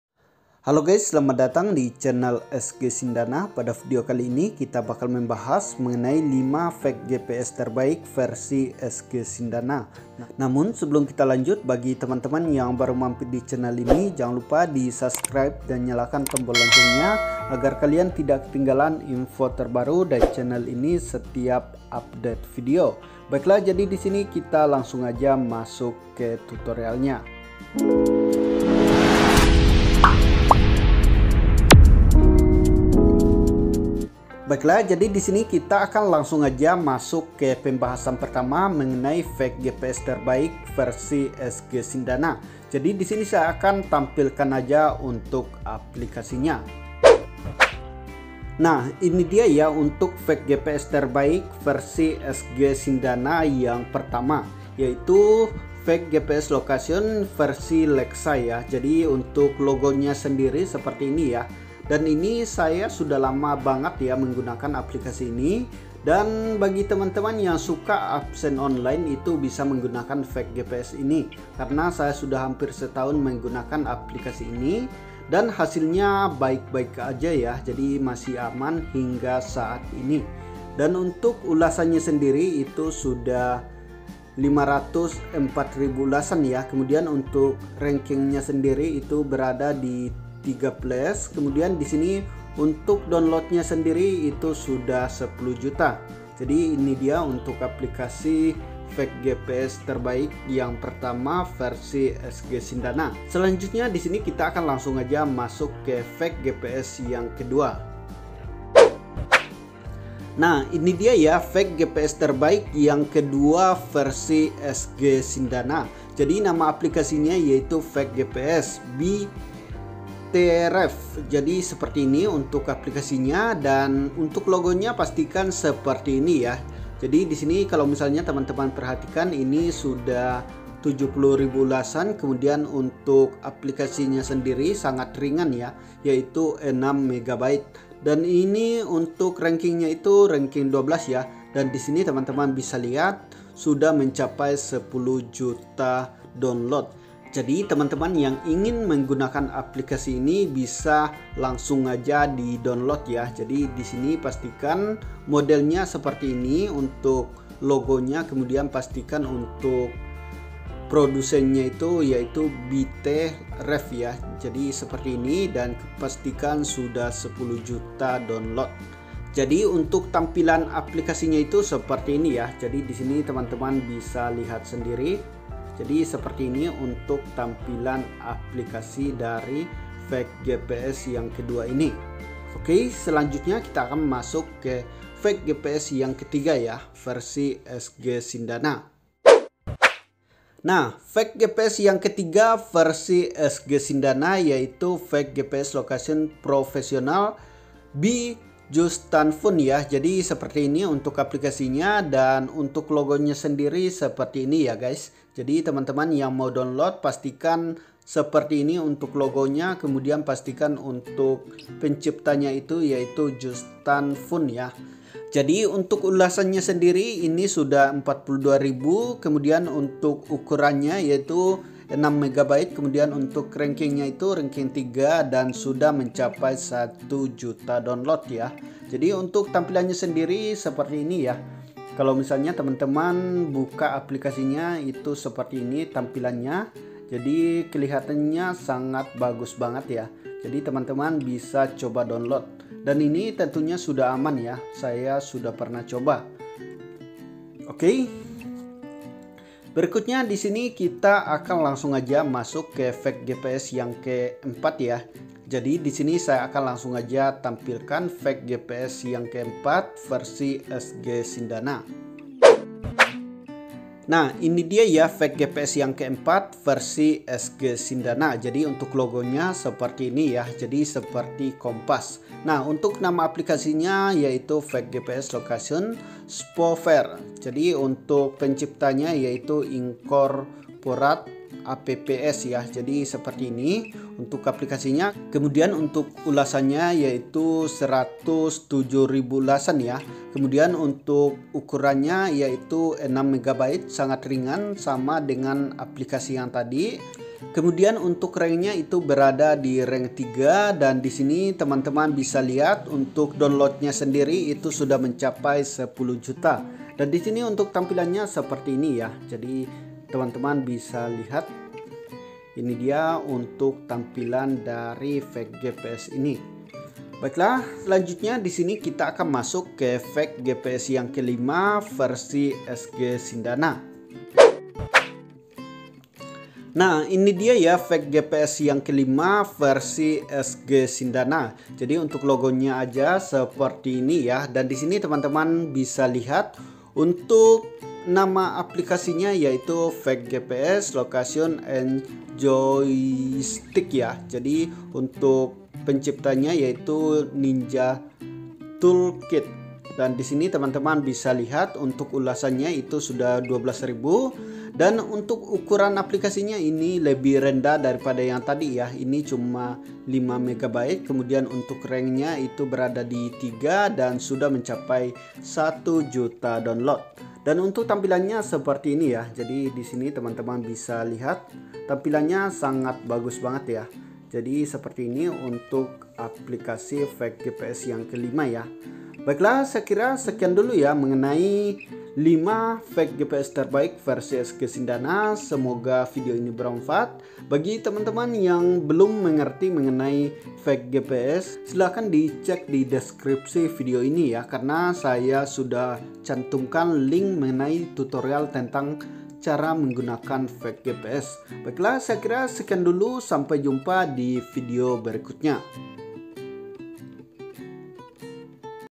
5 Fake GPS Terbaik Bagus Untuk Absen Online dan Game
Teman teman yang suka menggunakan fake gps, jangan kwatir, karena di video kali ini saya merekomendasikan 5 Fake GPS Terbaik Bagus Untuk Absen Online dan Game. Salah satu dari aplikasi di atas, masih saya gunakan hingga saat ini. Untuk lebih jelasnya silakan simak video di atas yah. Semoga bermanfaat.